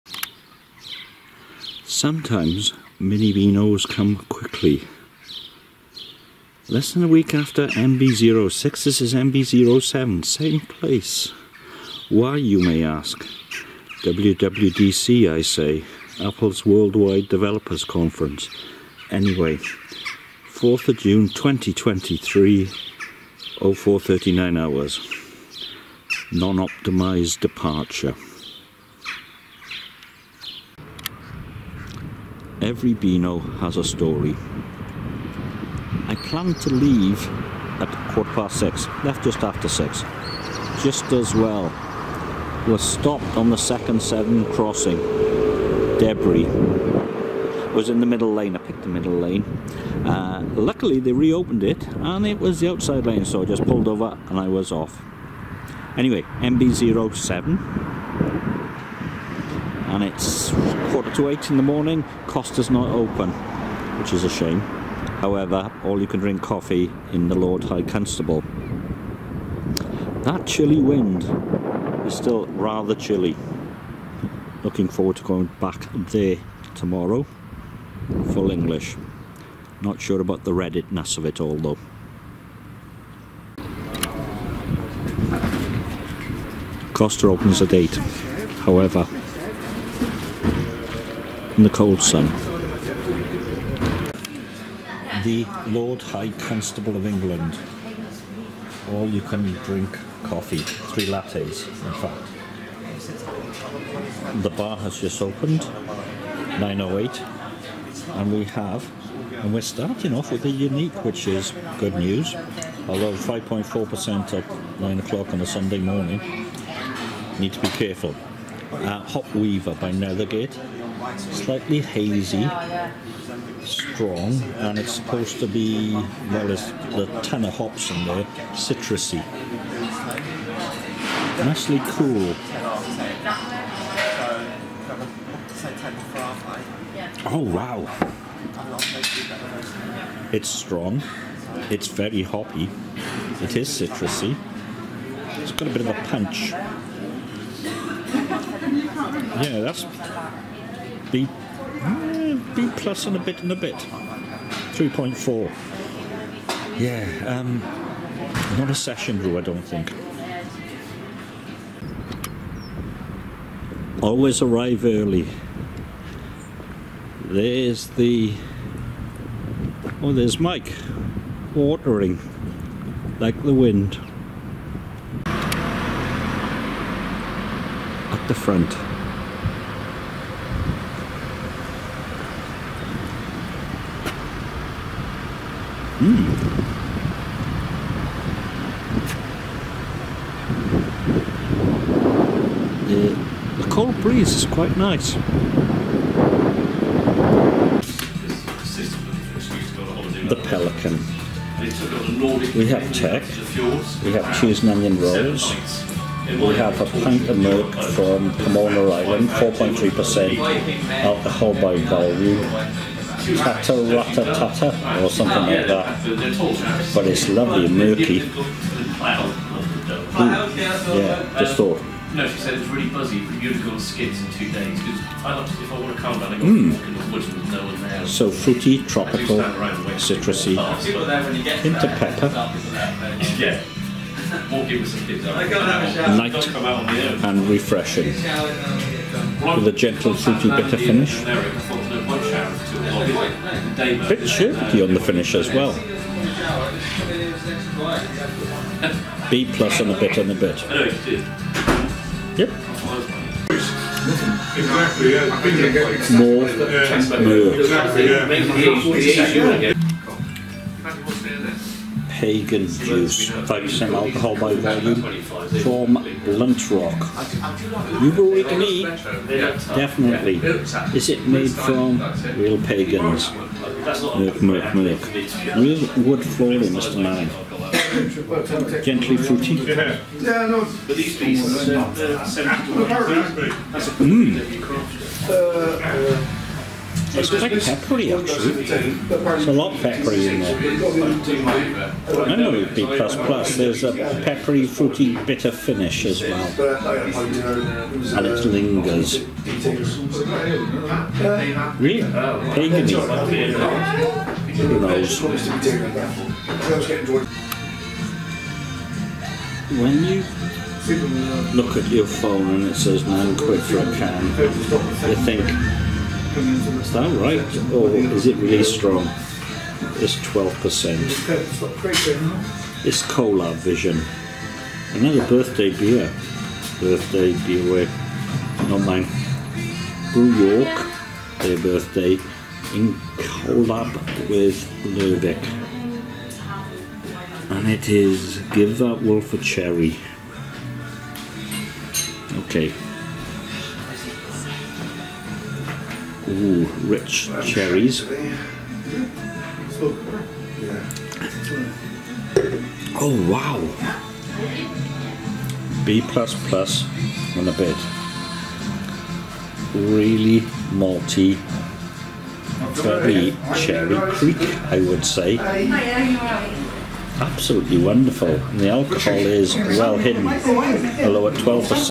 Debris On The Bridge
Leaving early, delayed and stopped on The Second Severn Crossing while a Policeman swept up Debris. Into The Lord High Constable and starting with an Unique. Cheese and Onion Rolls and a Pint of Murk to start in The Pelican. Another semi Birthday Ale, this time half Norwegian at 12% ABV.